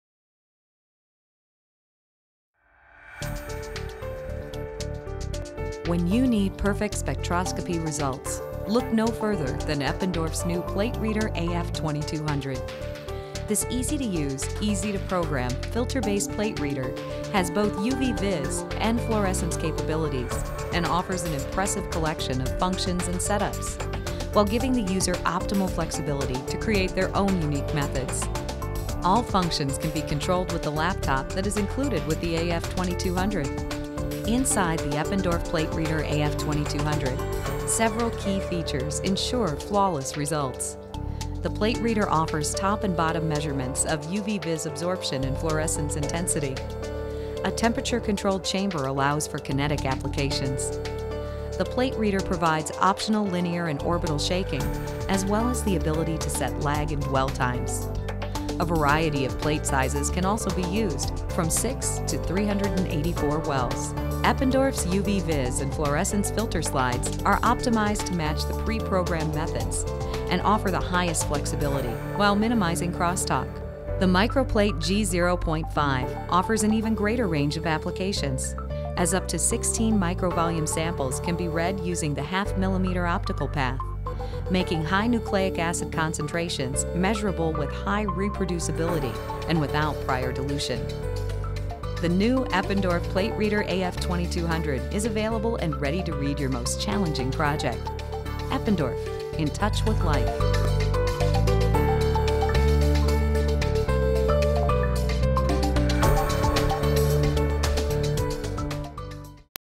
Eppendorf PlateReader AF2200, a Filter Based Plate Reader
The Eppendorf PlateReader AF2200 filter based plate reader has both UV/Vis and fluorescence capabilities, combined with pre-programmed methods. The plate reader offers top and bottom measurements of UV/Vis absorption and fluorescent intensity, a temperature controlled chamber, optional linear and orbital shaking, and can accommodate a variety of plate sizes, from 6 to 384 wells. Applications include nucleic acid and protein quantification, colorimetric protein assays, as well as cell-based and kinetic assays.